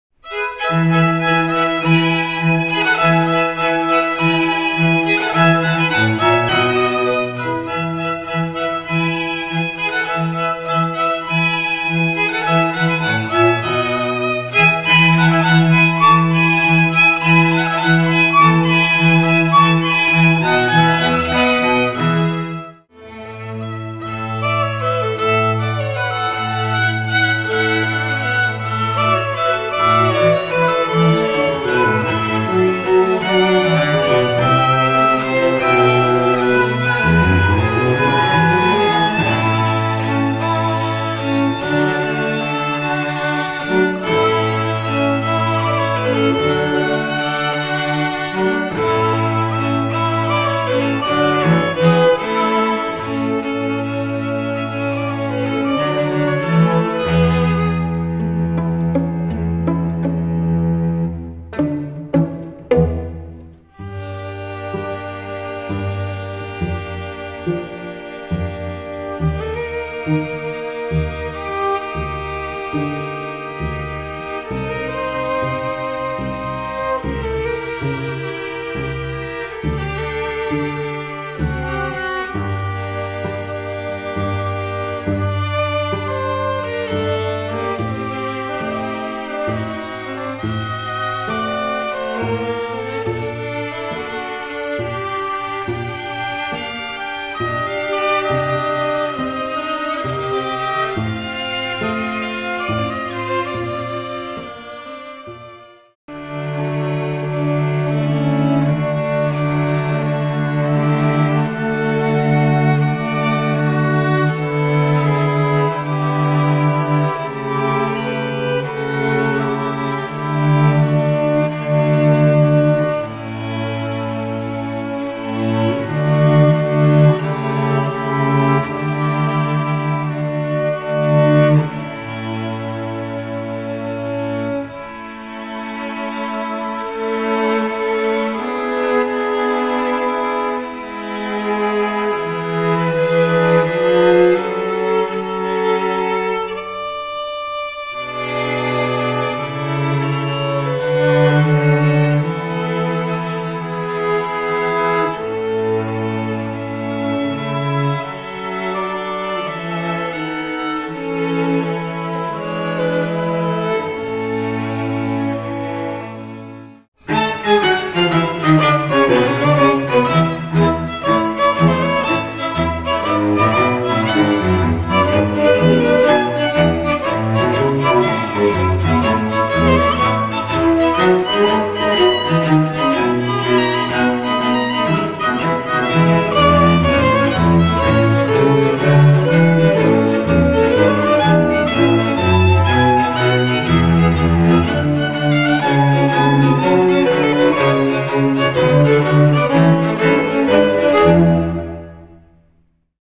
The Craxton String Quartet, recorded live, November 2017, Rowhedge, Essex, by Location Recordings.
The Craxton String Quartet Live ambient music for events, functions, occasions.